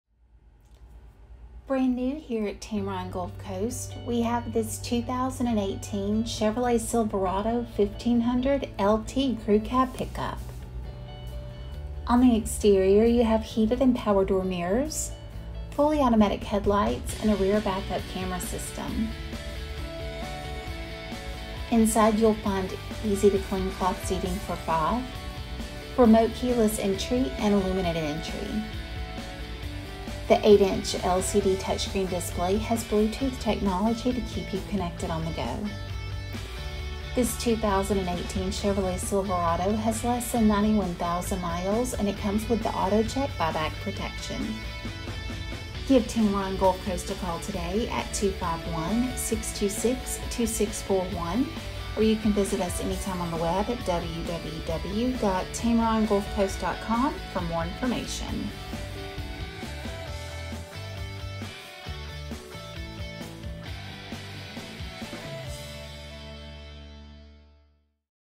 2018 Chevrolet Silverado 1500 LT Truck for Sale!!!
VIN: 3GCUKREC0JG598655
Stock: GM98655

Brand new here at Tameron Gulf Coast, we have this 2018 Chevrolet Silverado 1500 LT Crew Cab Truck!!
On the outside, you have heated and power door mirrors, fully automatic headlights, and a rear back up camera.
Inside you will find easy to clean cloth seating for five, remote keyless entry, and Illuminated entry.
The 8 inch LCD touchscreen display has Bluetooth technology to keep you connected on the go.
This 2018 Chevrolet Silverado 1500 less than 91,000 and comes with The AutoCheck Buyback Protection.